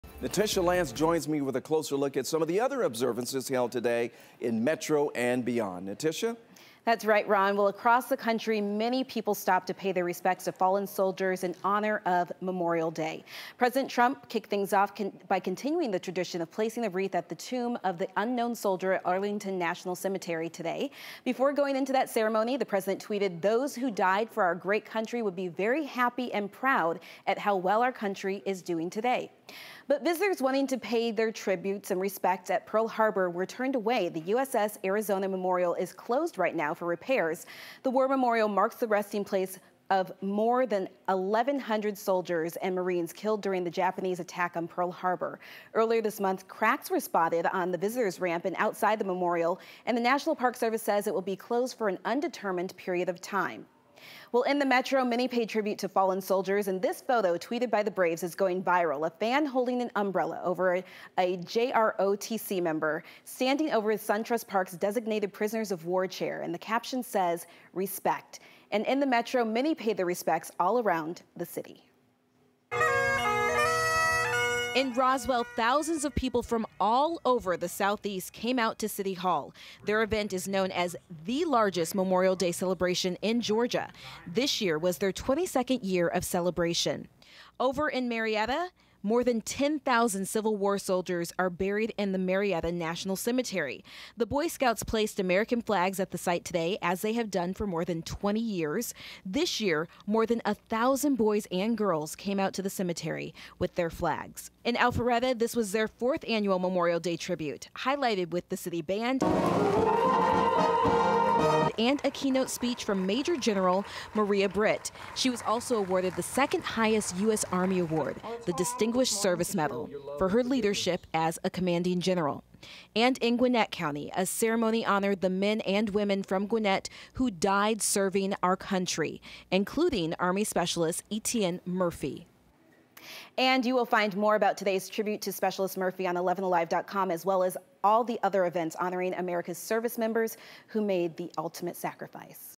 Remembering the fallen on Memorial Day
From the metro area and beyond, here's how the country marked the day.